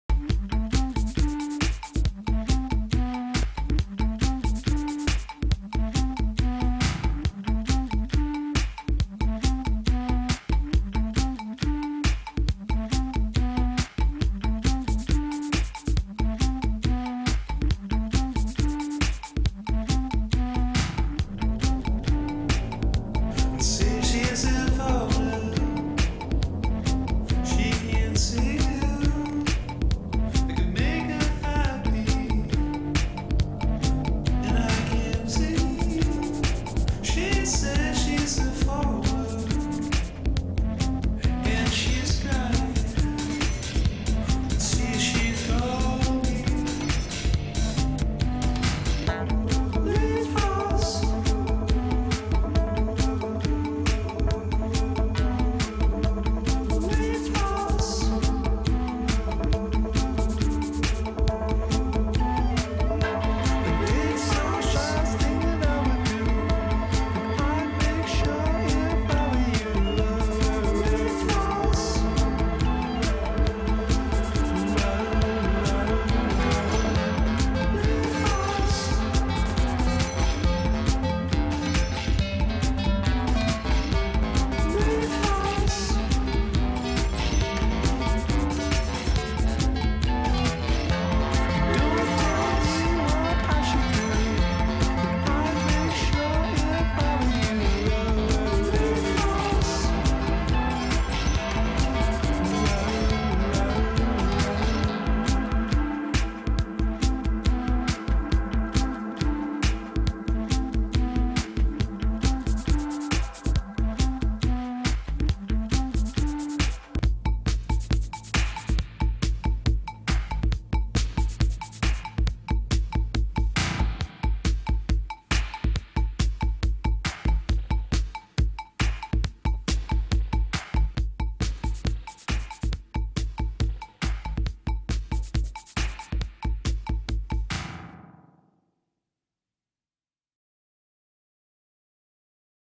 6 hour full pack move in just 2 minutes
A great big thank you to Matt Durning for sending us this amazing video of his local move with NorthStar Moving, documenting the whole 6 hours full packing move in just 2 minutes! beautifully done! you can check out more of his work and check us out on the web